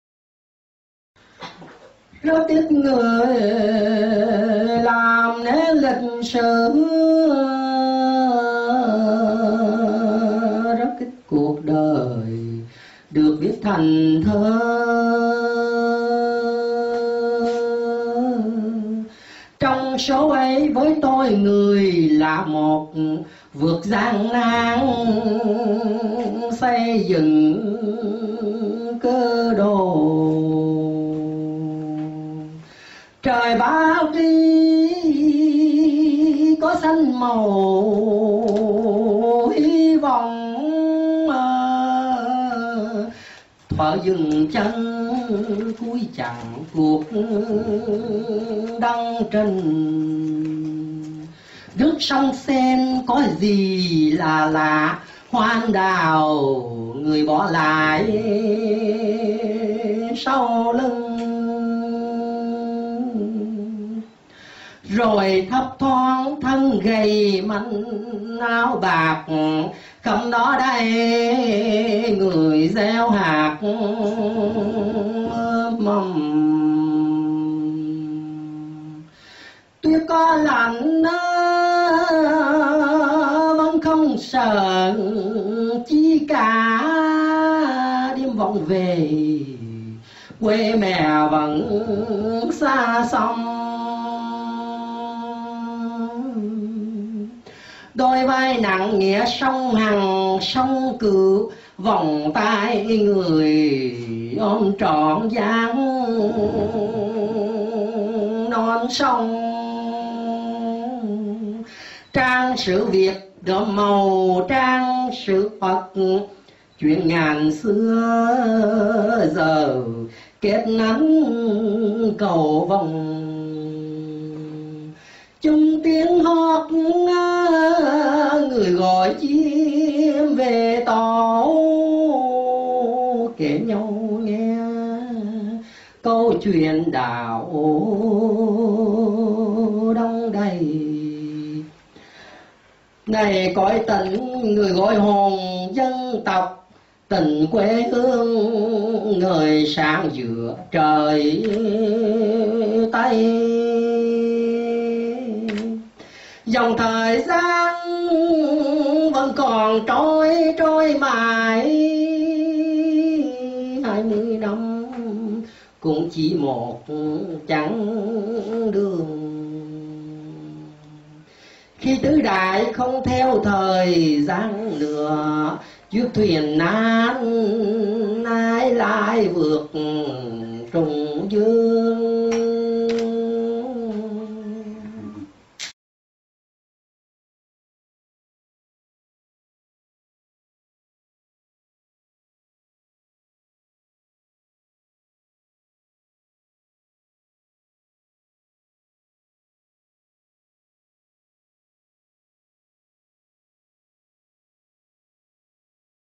HT. Thích Kiến Tánh - Bài Thơ Dâng Người
Bài Thơ Dâng Người

Rất ít người làm nên lịch sử
Rất ít cuộc đời được viết thành thơ
Trong số ấy, với tôi, người là một
Vượt thời gian, người xây dựng cơ đồ.

Trời Paris có xanh màu hy vọng?
Thuở dừng chân cuối chặng cuộc đăng trình
Nước sông Sein chắc có gì là lạ
Hoa Anh Đào người bỏ lại sau lưng.

Rồi thấp thoáng thân gầy, manh áo bạc
Khắp đó đây người gieo hạt, ươm mầm
Tuyết có lạnh, vẫn không sờn chí cả
Đêm vọng về, quê mẹ vẫn xa xăm.

Đôi vai nặng nghĩa sông Hằng, sông Cửu
Vòng tay người ôm trọn dáng non sông
Trang sử Việt đượm màu trang sử Phật
Chuyện ngàn xưa giờ kết ánh cầu vồng.

Chung tiếng hót, người gọi chim về tổ
Kể nhau nghe câu chuyện Đạo đong đầy
Đây cõi Tịnh người gọi hồn dân tộc
Tình quê hương ngời sáng giữa trời Tây.

Dòng thời gian vẫn còn trôi, trôi mãi.
Hai mươi năm cũng chỉ một chặng đường
Khi tứ đại không theo thời gian nữa
Chiếc thuyền nan ai lái vượt trùng dương?

Kính Tặng Giác Linh
HT. Thích Minh Tâm - Khánh Anh - Pháp Quốc

HT. Thích Kiến Tánh Đề Thơ
ĐĐ. Thích Pháp Trú Diễn Ngâm